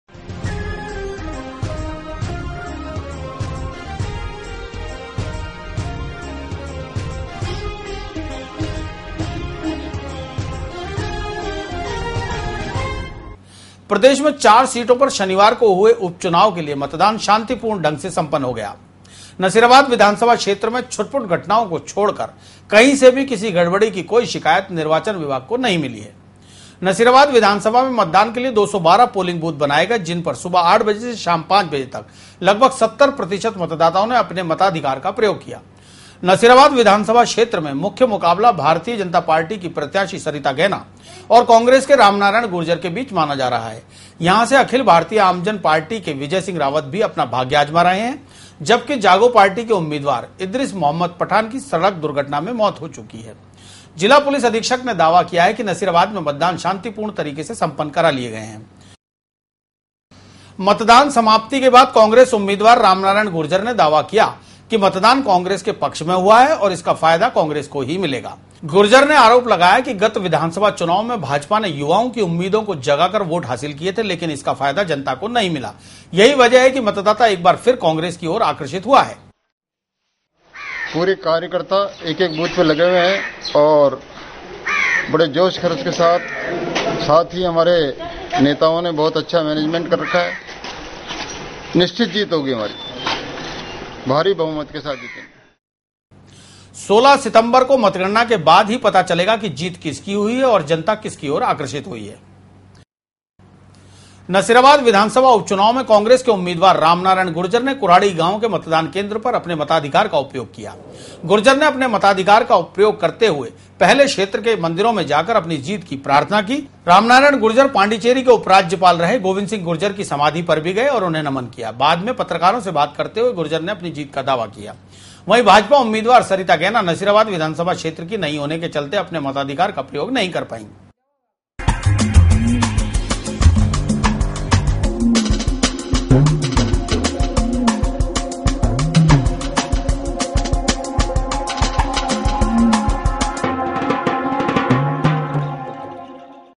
नसीराबाद विधानसभा उपचुनाव में 70 प्रतिशत हुआ मतदान।
प्रदेश की 4 विधानसभा सींटों पर हुए उपचुनाव शांतिपूर्ण तरीके से सम्पन्न। नसीराबाद विधानसभा क्षेत्र में छुटपुट घटना को छोड़कर लगभग 70 प्रतिशत हुआ मतदान।